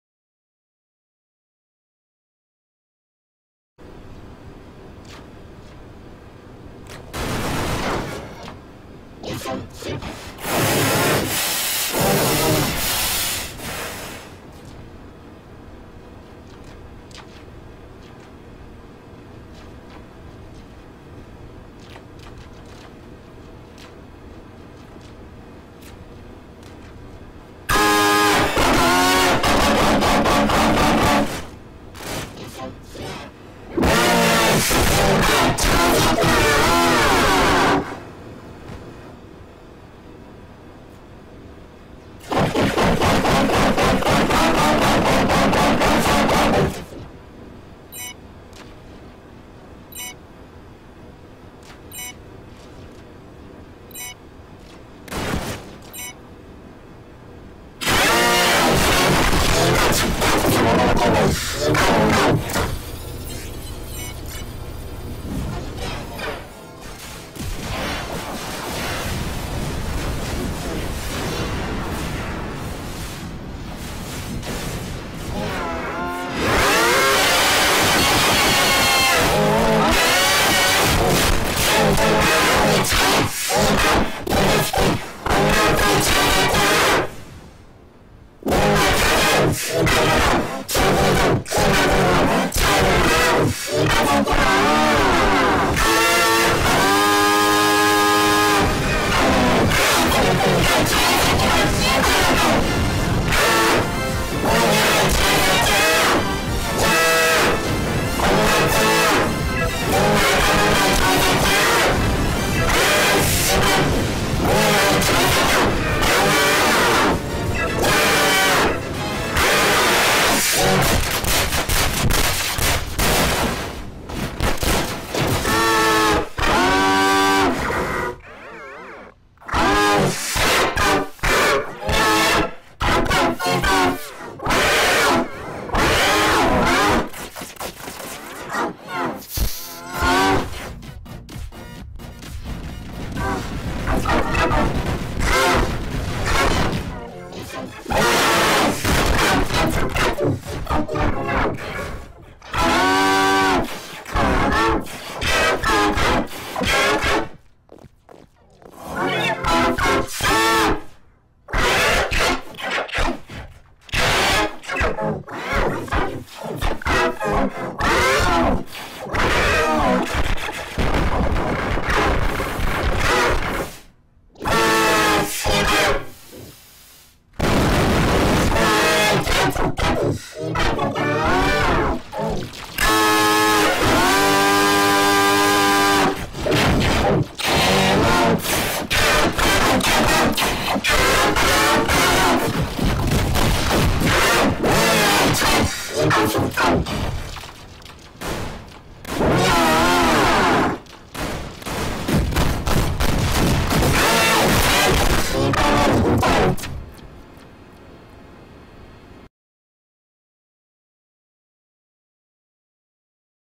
Haha This is Angry Korean Gamer with G-Major pitched voice!!!😈😈😈
Look Officer Anthony I send you a video is Angry Korean Gamer with G-Major pitched voice so this is for you Anthony and I will see you tomorrow good night 🌙💤